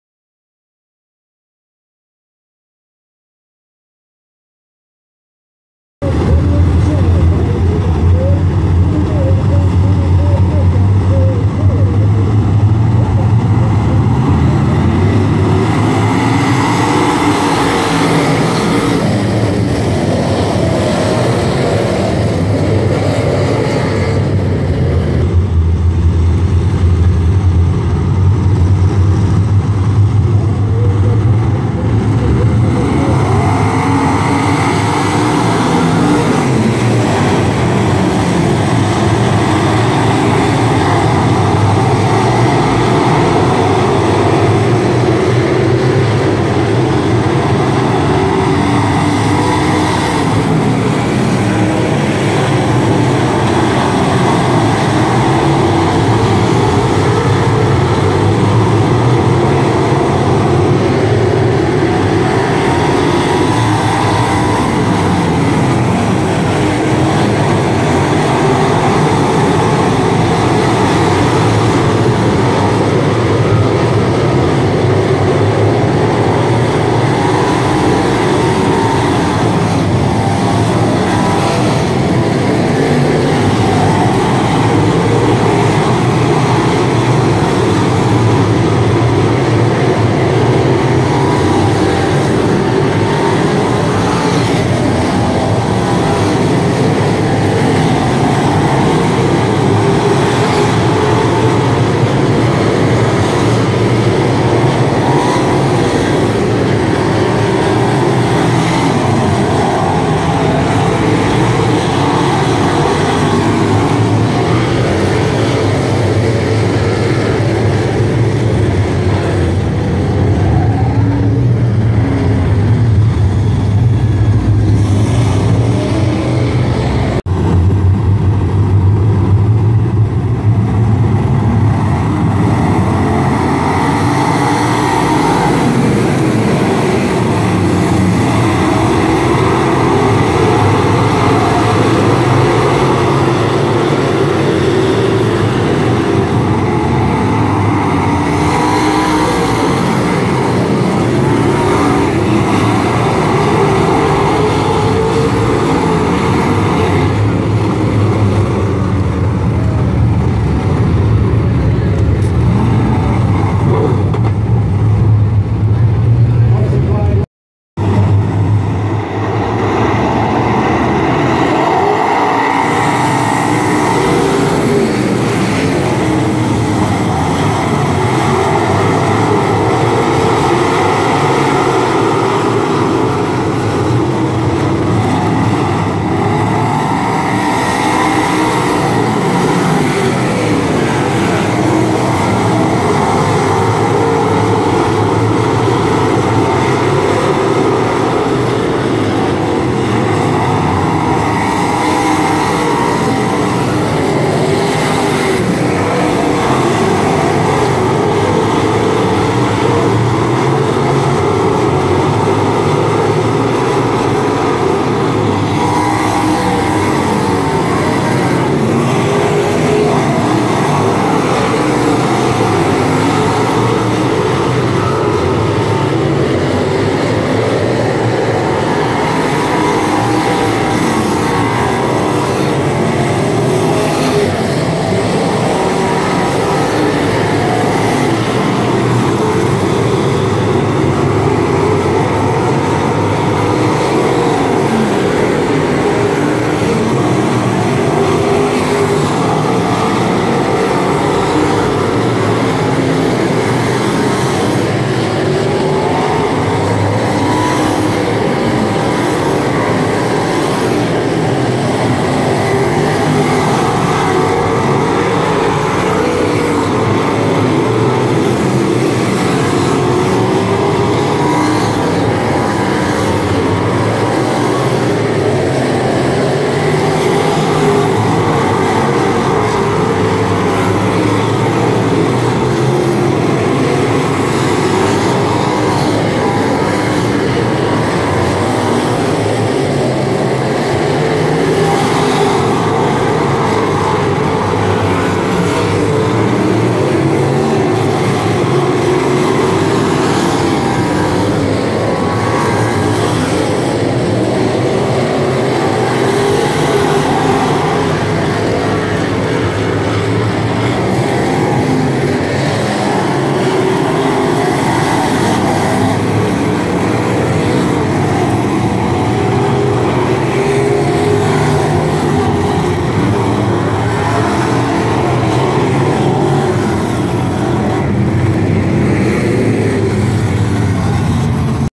Sportsman Feature @ Wartburg Speedway (7-3-17)
Sportsman Late Model Feature @ Wartburg Speedway (7-3-17)
Darren Goins D2 Win